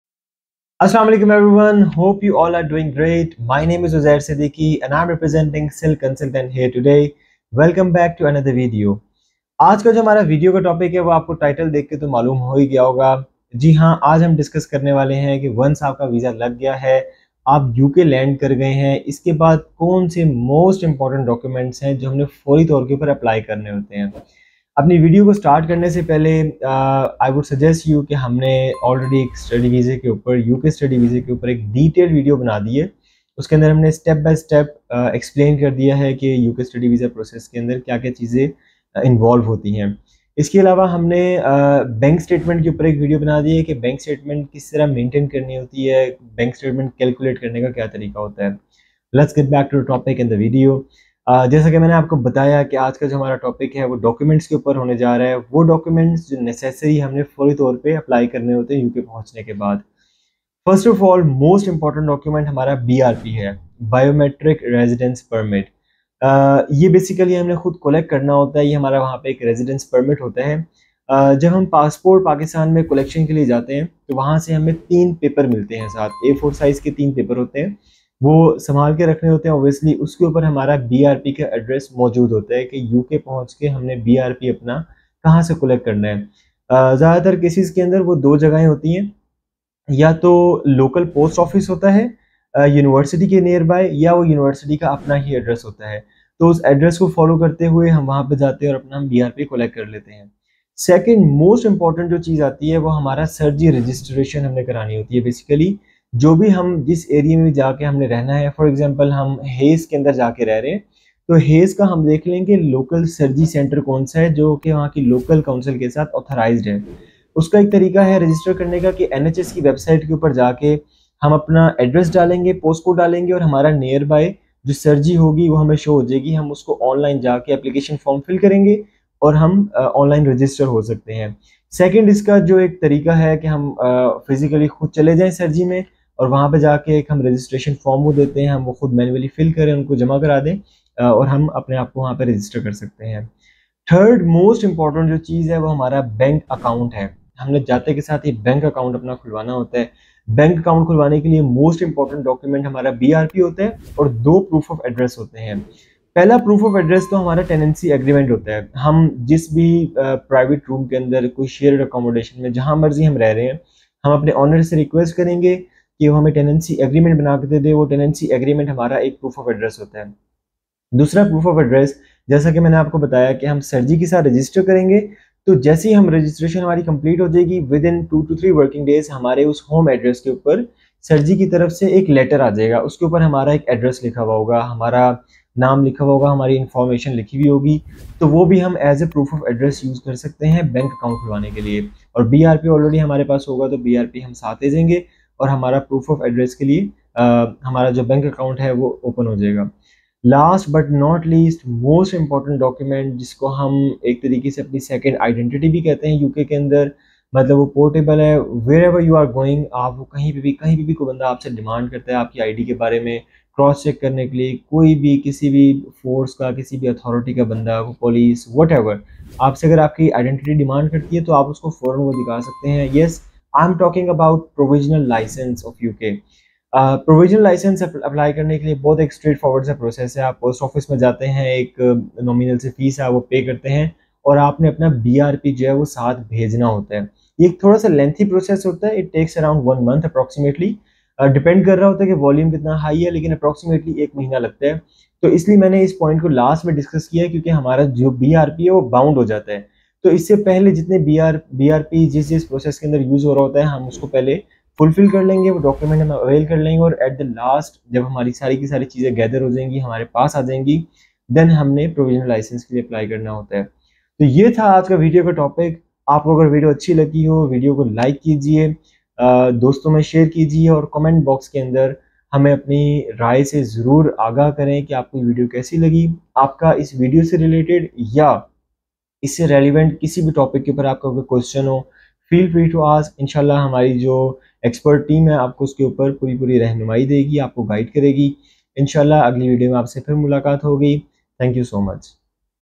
THINGS TO DO AFTER ARRIVING IN THE UK || STUDY IN UK
If you've just arrived in the UK to study, this video is for you! Learn about the important things to do after arriving in the UK as a student. From setting up a bank account to exploring your new surroundings, we've got you covered!